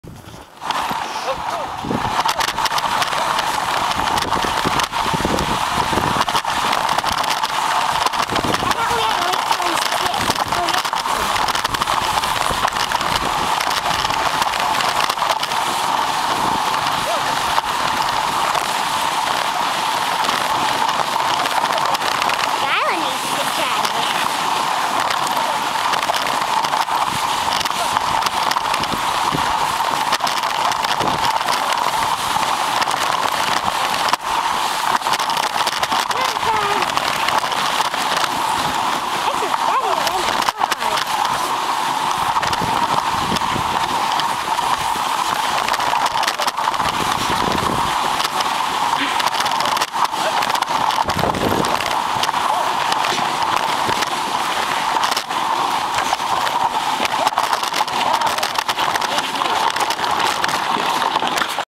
Tundra sled training
Here's our Tundra giant Malamute at 16 months pulling a sleigh, in training. Pulling daughter Cheyanne in Arnprior Ontario December 30, 2012